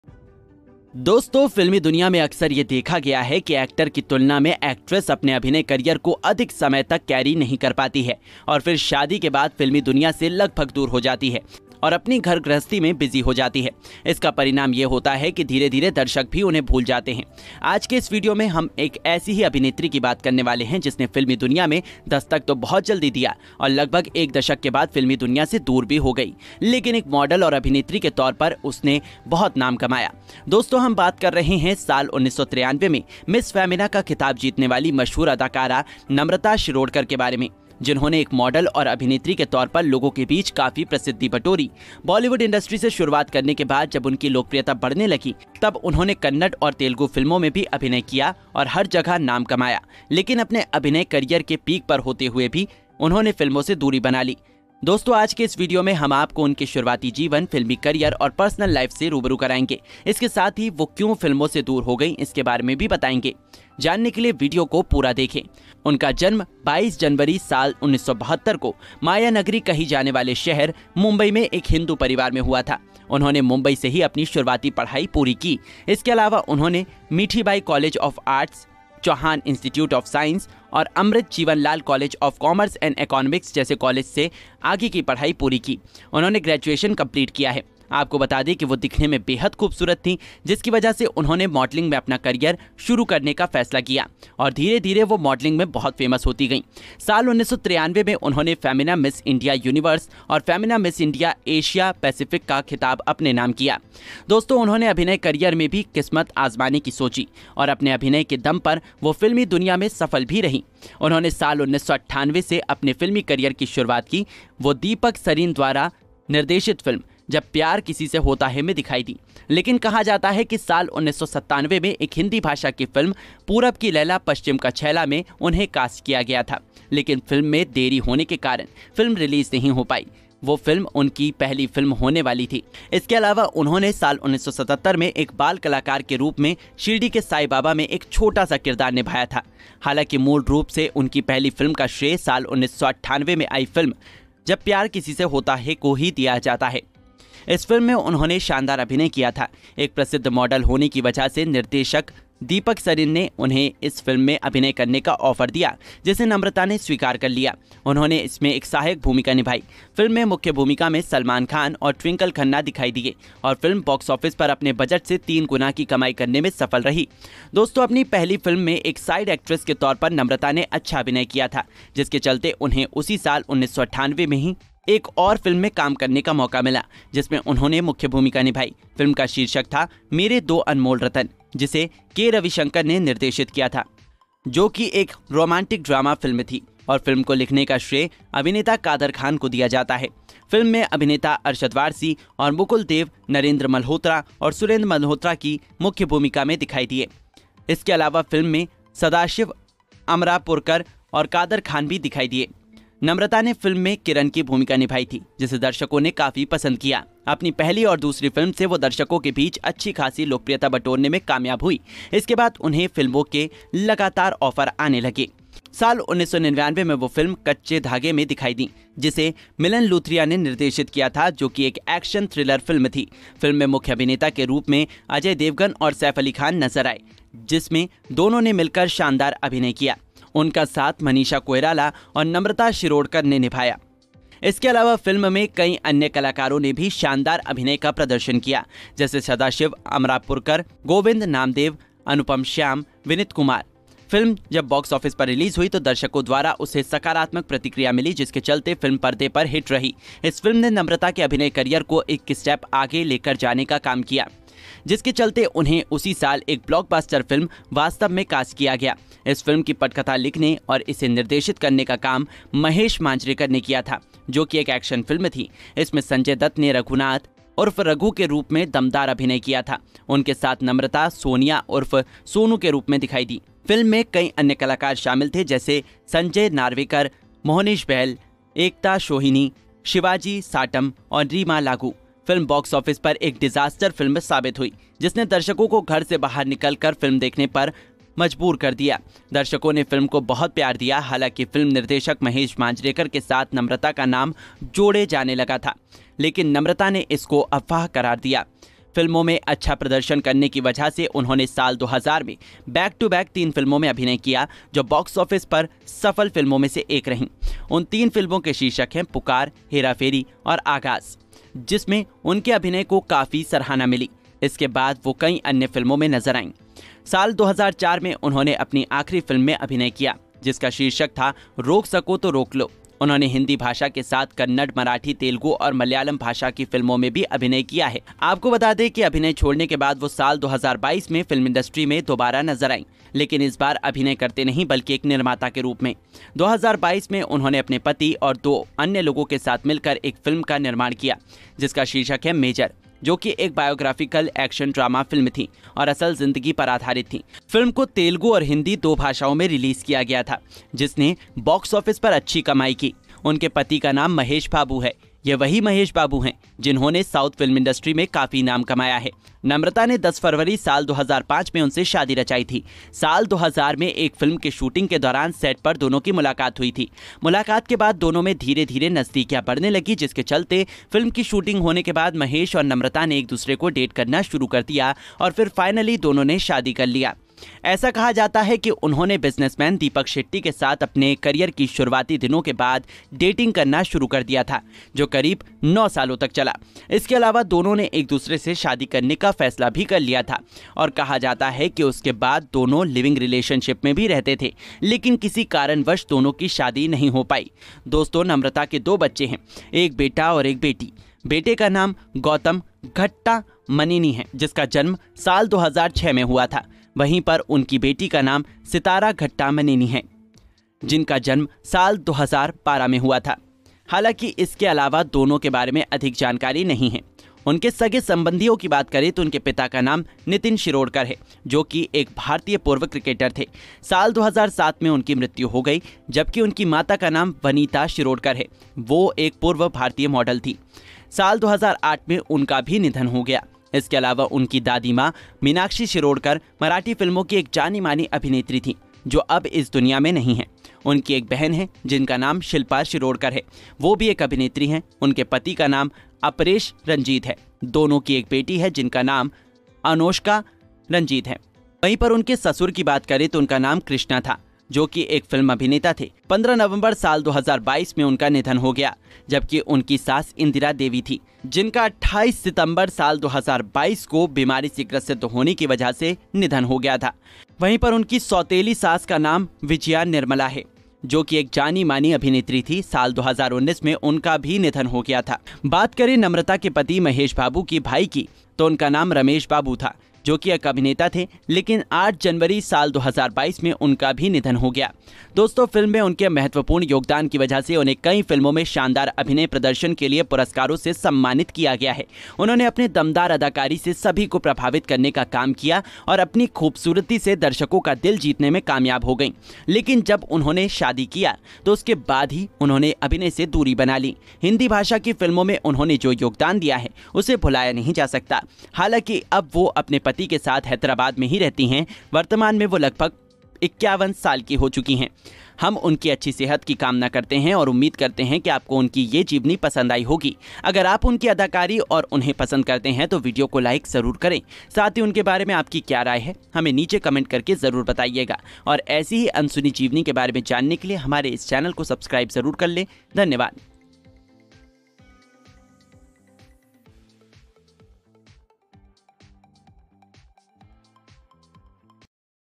Vaastav Movie Actress Namrata Shirodkar With Her Father-in-Law, Husband, and Children | Biography
Vaastav Movie Actress Namrata Shirodkar With Her Father-in-Law, Husband, and Children | Parents | Biography

Actress Namrata Shirodkar Family, Biography in Hindi